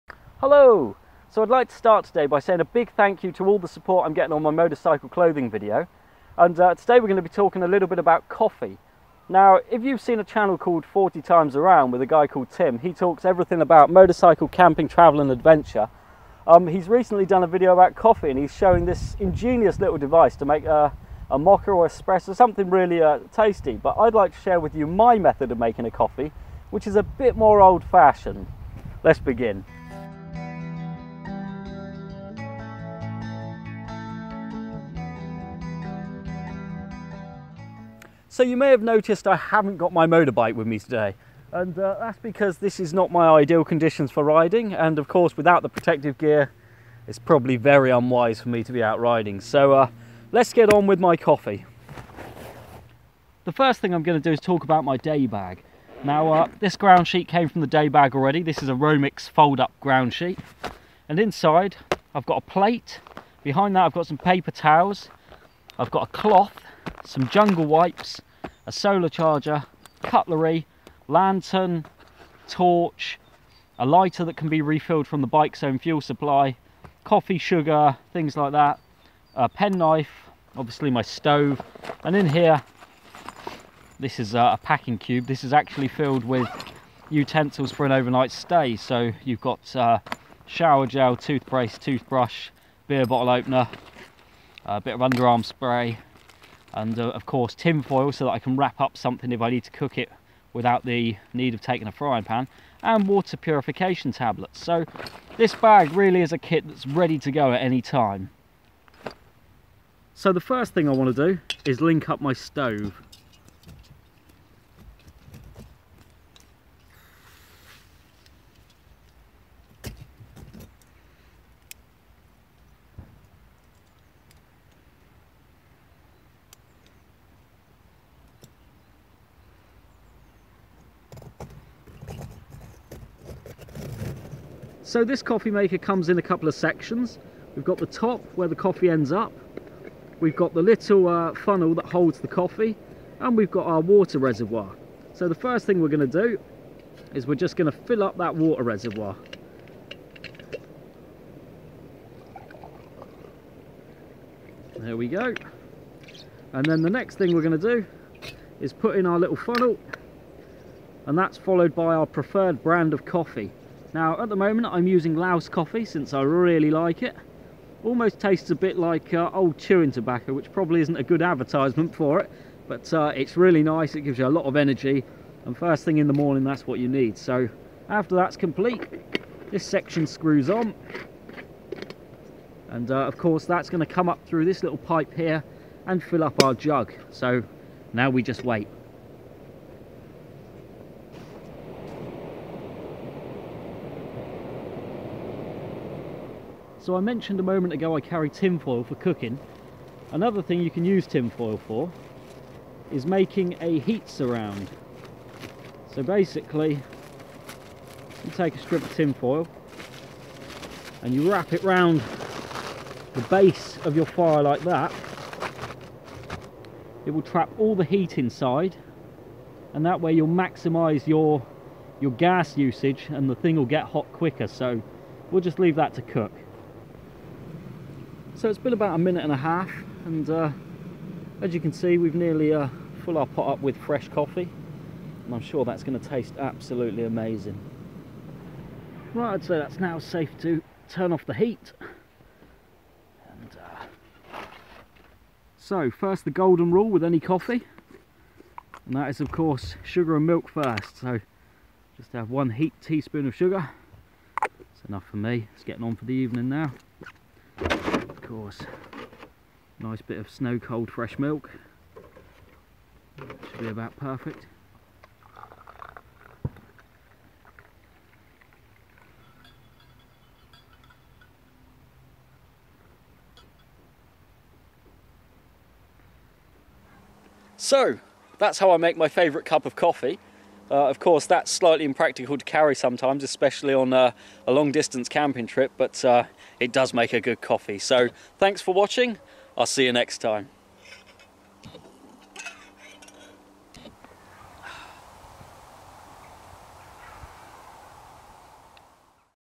My Perfect Coffee when Camping
I always start my day with a nice fresh cup of coffee, and today I will be showing you how I make my perfect coffee when I'm out camping. Unfortunately due to space I normally take instant coffee, but you can't beat a nice full flavoured traditional coffee.